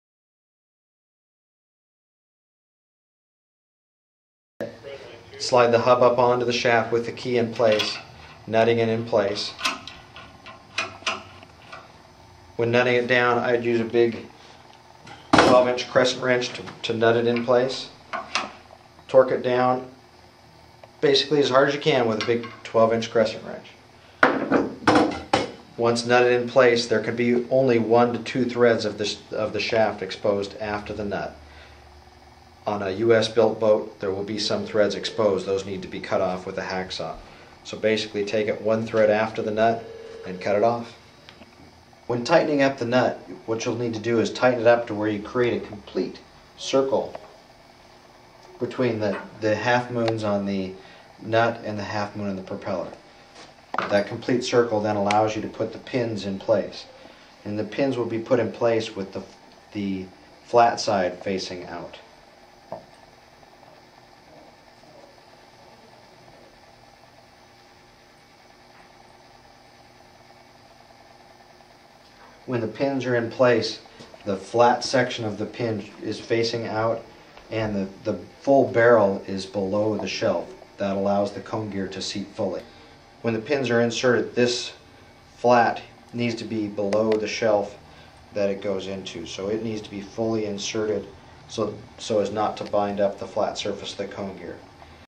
Max-Prop: Nut and pin installation on a feathering propeller
On this feathering 2 Blade propeller from Max-Prop we show you how to properly attach the nut and insert the pins.